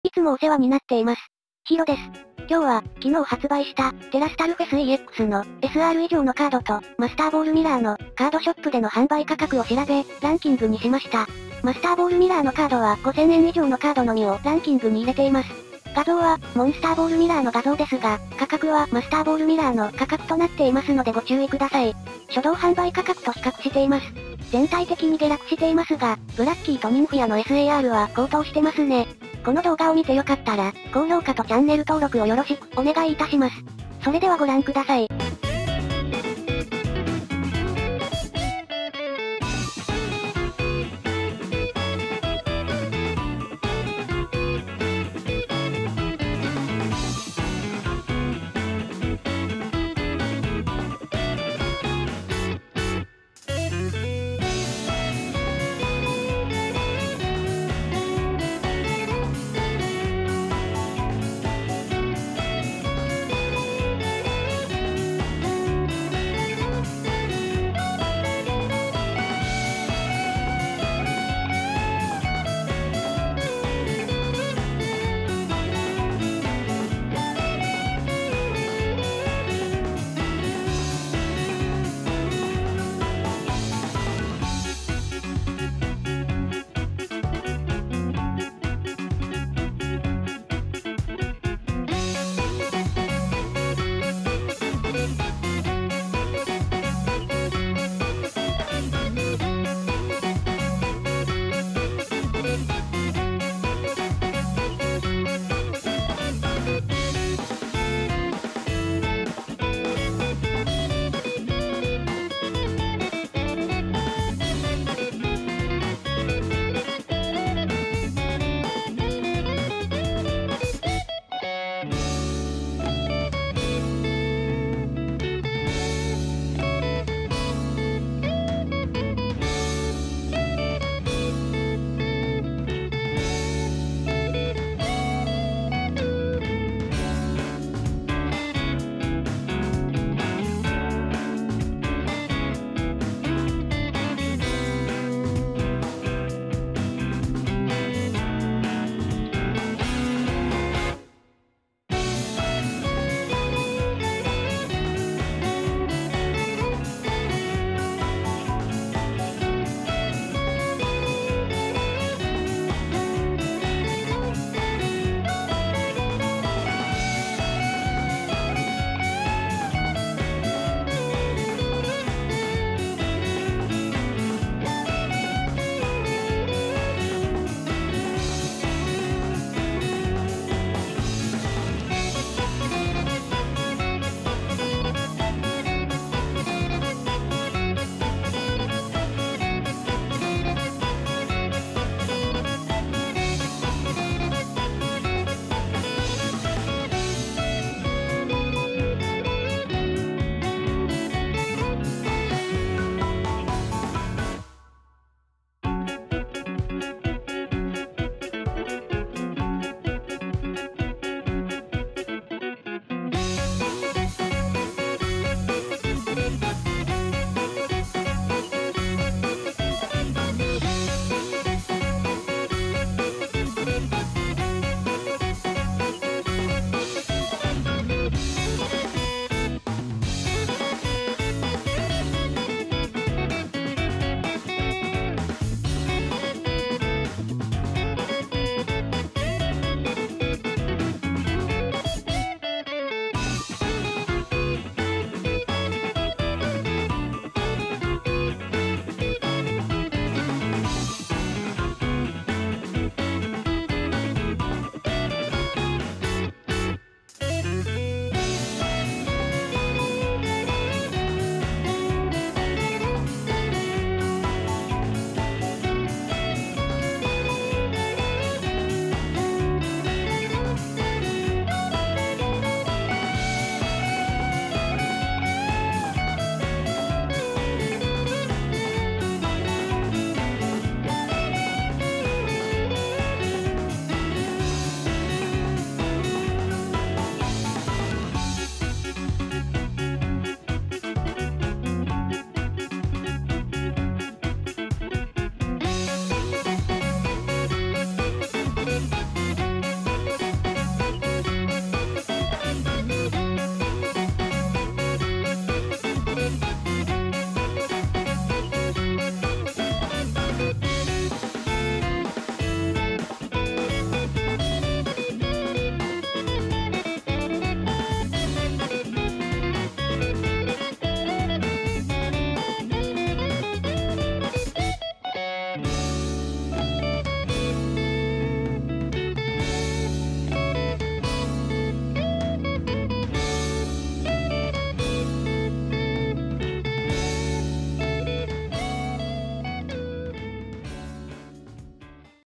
【ポケカ】ブラッキー高い！発売から1日後！販売価格ランキング テラスタルフェスex SR以上+α【Pokémon cards】Market price of Prismatic Evolution
■コメント
いつもお世話になっています。Hiroです。
今日は、昨日発売した、テラスタルフェスexの、SR以上のカードと、マスターボールミラーの、
カードショップでの販売価格を調べ、ランキングにしました。
マスターボールミラーのカードは5000円以上のカードのみをランキングに入れています。
画像は、モンスターボールミラーの画像ですが、価格はマスターボールミラーの価格となっていますのでご注意ください。

初動販売価格と比較しています。
全体的に下落していますが、
ブラッキーとニンフィアのSARは高騰してますね。

この動画を見てよかったら、

※カードラッシュ様の販売価格を参考にしています。

今後はこちらにも投稿していきますのでご登録をよろしくお願いいたします！

チャンネルのコンセプトは「どこでも、すぐに見られる動画」です。
横スクロールのポケカの相場動画を投稿し始めたのは私が最初です。